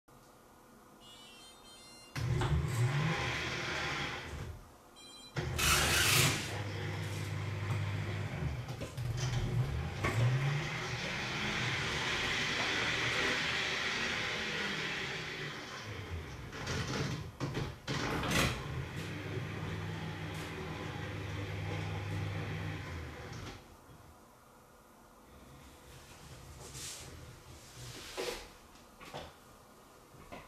車庫入れ 1
位置・姿勢マーカー(カラーボール2個)を認識して所定の位置・姿勢に移動させるテスト．
区画線のはみ出しあり．